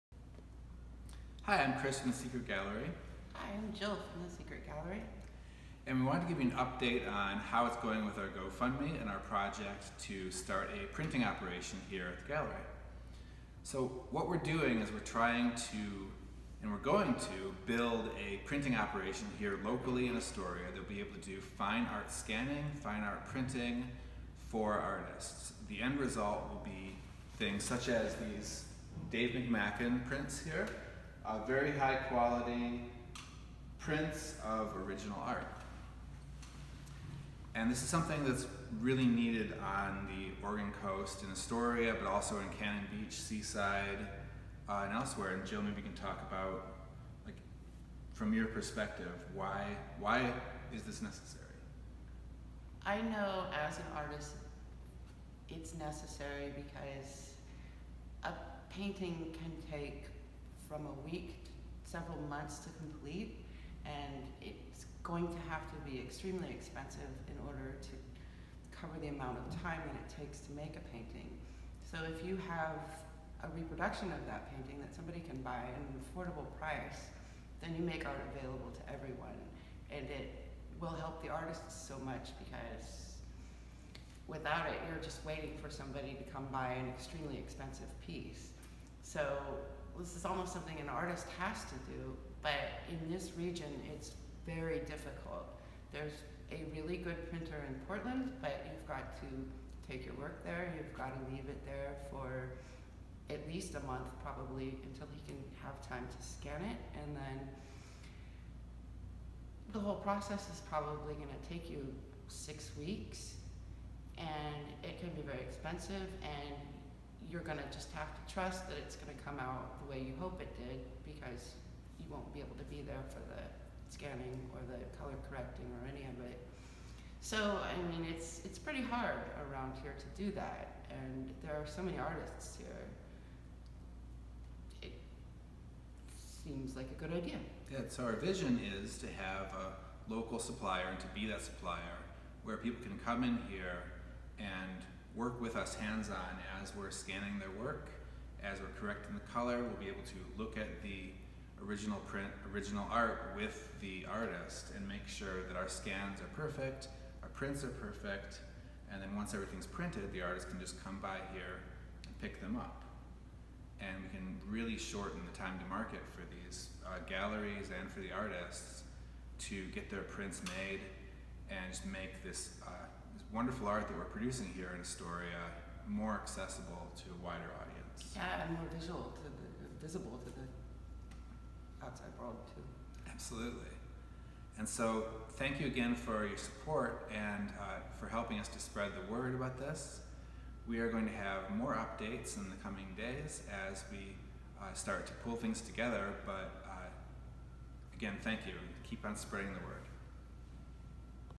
The Secret Gallery Needs a Printer - Update #1
We need a printer, and here's why, and here's how you can help!